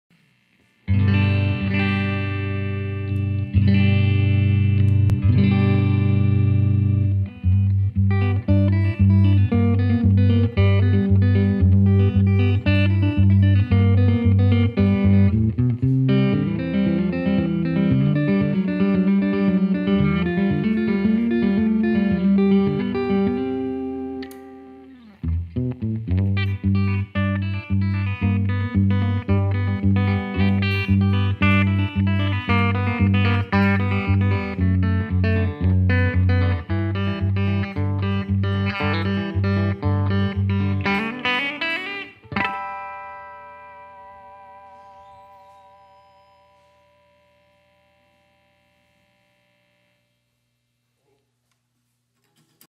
Snacks 002: Gibson Memphis '61 ES-330TD Figured VOS
Russo Music Snacks: Quick bites of our favorite new gear. Still hungry? Wills Weller (actually our resident drum nerd) gets out the fingerstyle on this figured top full-hollow screamer from the fine folks at Gibson Memphis. P-90s and bigsby round out the package, how can you go wrong with that.
2016 Gibson Memphis1961 ES-330TD Figured Vintage Sunburst VOS, 6lbs 13oz, serial- Q11206
Mics: Sennheiser 421 with an AKG C451 B on the room.